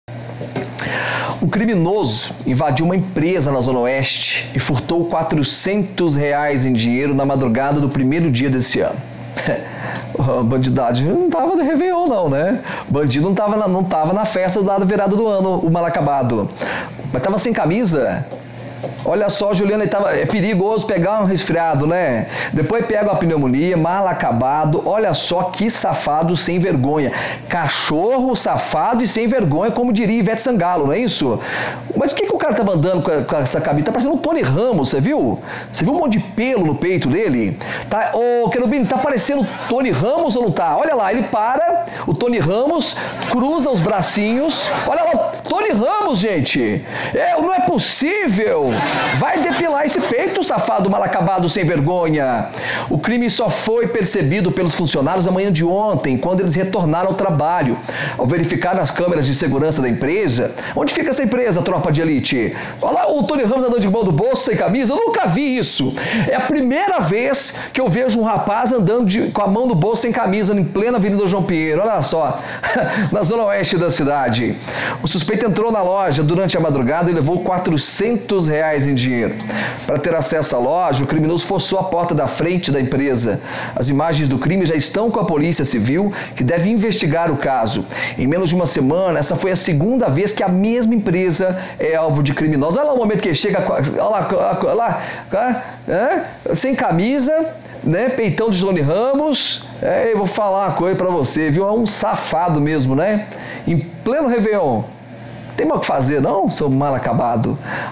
LOJA INVADIDA
CRIMINOSO INVADE EMPRESA E ROUBA R$ 400 REAIS

Uma loja foi invadida na Zona Oste de Poços de Caldas na madrugada do primeiro dia do ano, o criminoso levou R$ 400 reais em dinheiro, em menos de uma semana é a segunda vez que a loja é alvo de furto.